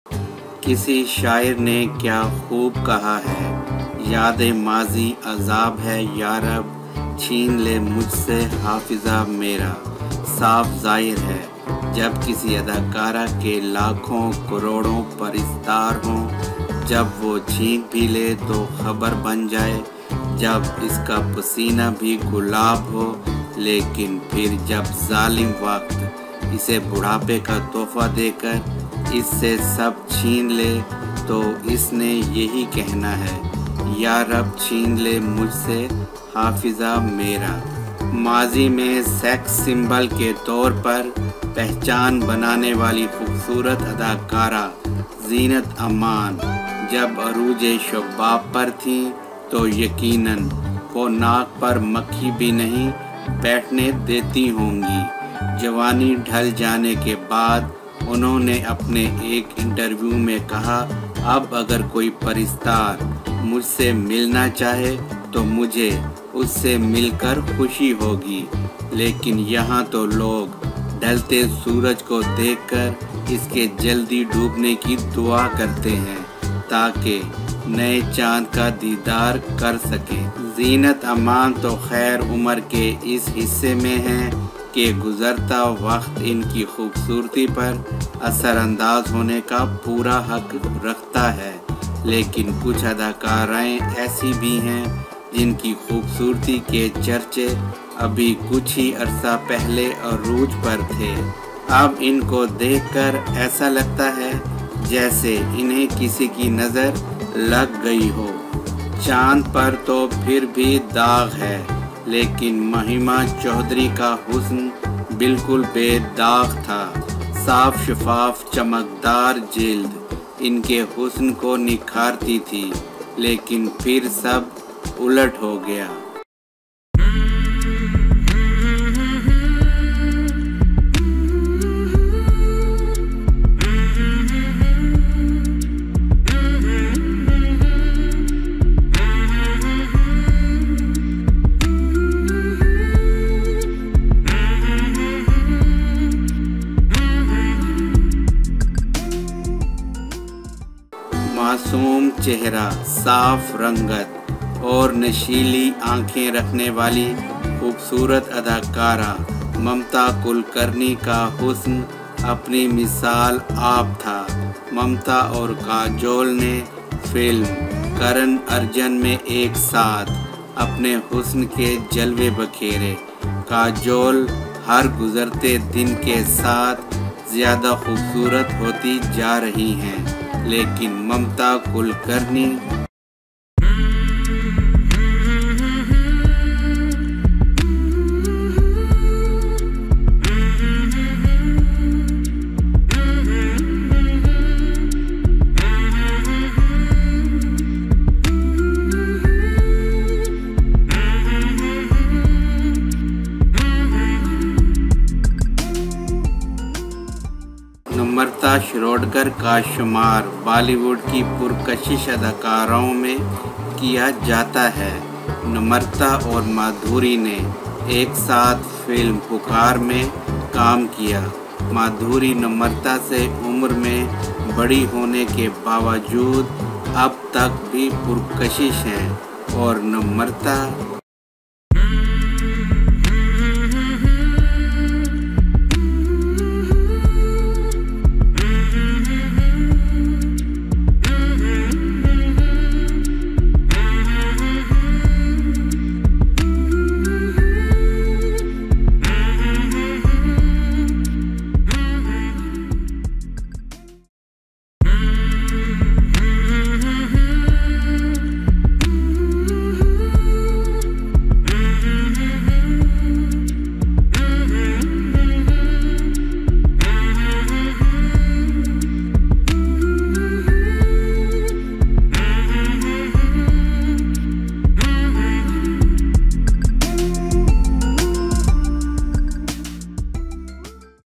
Pretty Heroines Lose Beauty Before Time || Mamta Kulkarni || Mahima Chaudhry || Namrata Shirodkar
Mamta Kulkarni
Born: April 20, 1972 (age 47 years), Mumbai, India
Height: 1.58 m
Spouse: Vicky Goswami (m. 2013)
Mahima Chaudhry
Born Ritu Chaudhury
13 September 1973 (age 45)
Darjeeling, West Bengal, India[1]
Occupation Actress
Years active
1997–2010
2015–2016
Spouse(s) Bobby Mukherjee
(m. 2006; div. 2013)
Children 2
Parents: Mukund Kulkarni
Namrata Shirodkar
Residence Film Nagar, Hyderabad, India
Occupation Actress, model
Years active 1993–2004
Title Femina Miss India Universe 1993
Femina Miss India Asia Pacific 1993
Spouse(s) Mahesh Babu
(2005 – present)
Children 2
Beauty pageant titleholder
Major
competition(s) Femina Miss India Universe 1993
(Winner)
(Winner)
Miss Universe 1993
(Top 6)
Miss Asia Pacific 1993
(1st Runner-up)
(Best in Evening Gown)